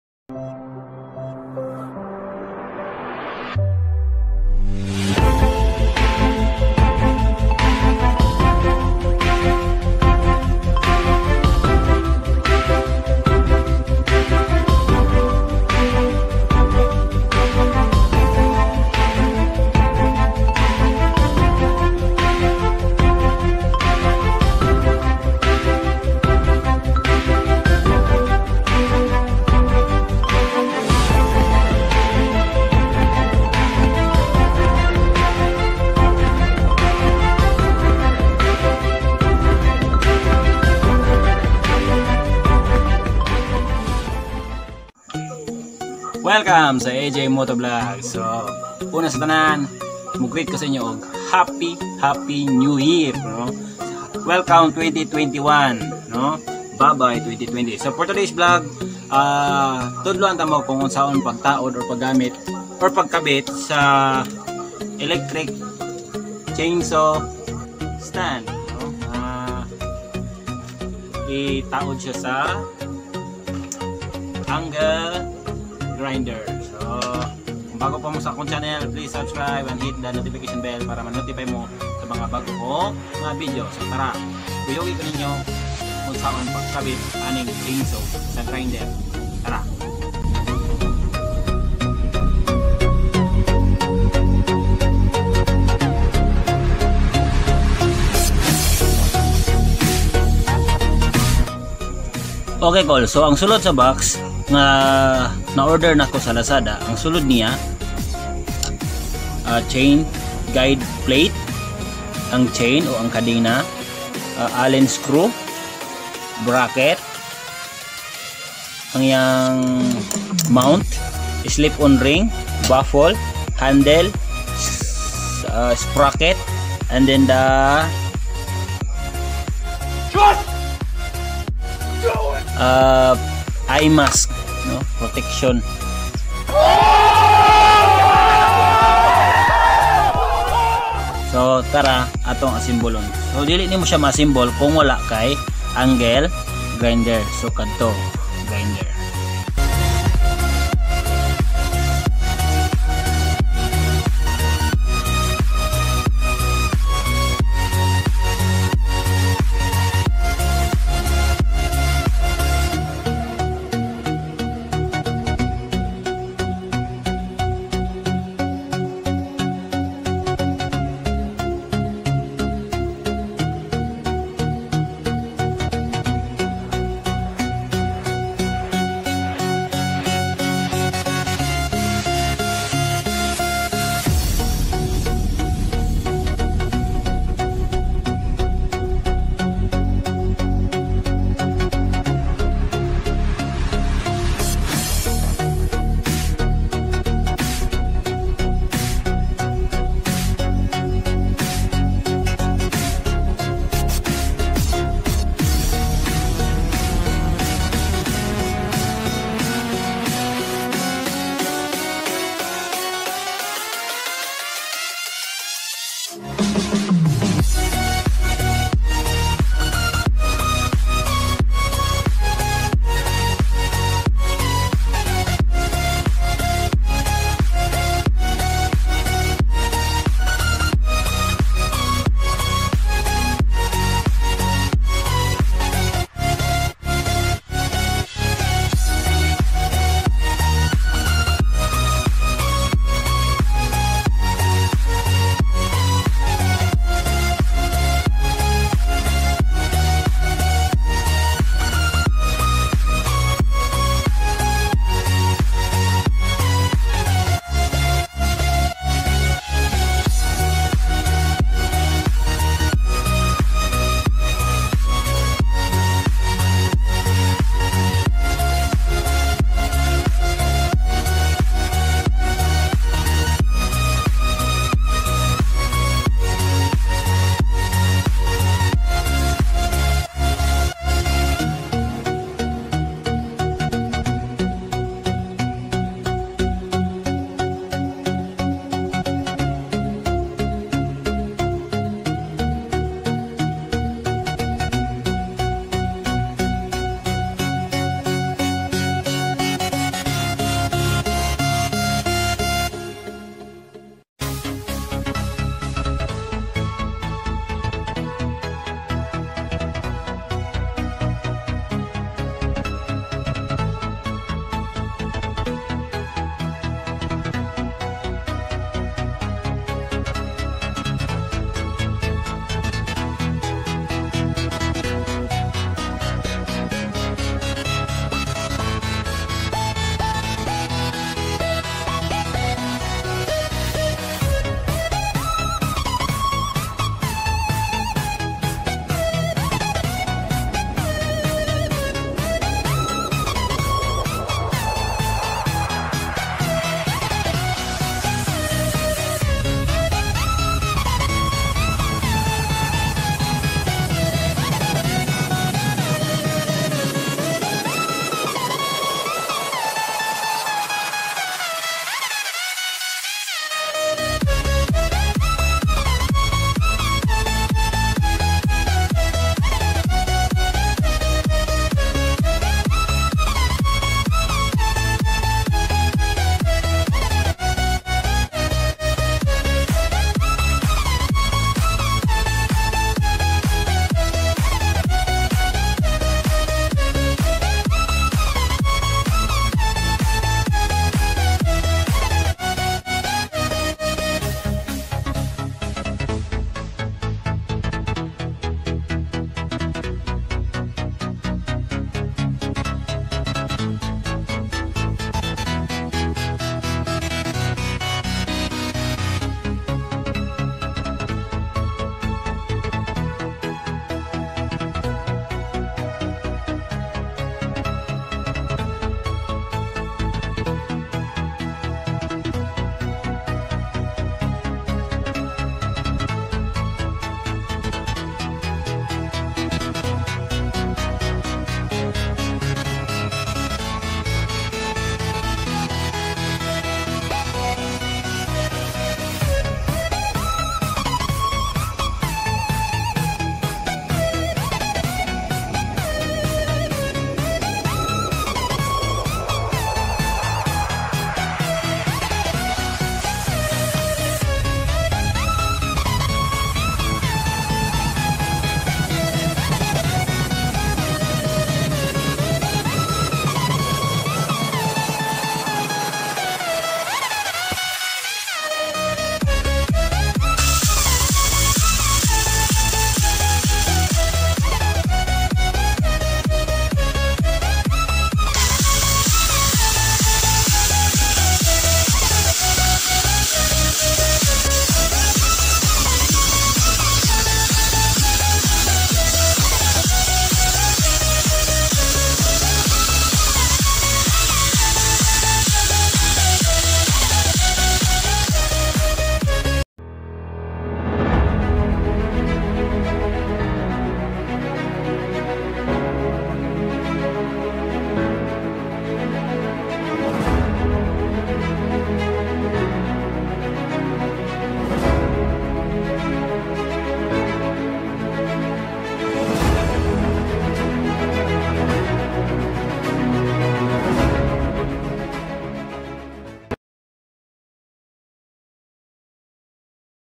Installation of Chainsaw Adapter for Grinder
tutorial video po to mga angkol at mga antie kung pano e install ang chainsaw adaptor for grinder.
pls magcomment lang po at handa po ako makinig o magbasa sa inyong mga comments.
happy new year po.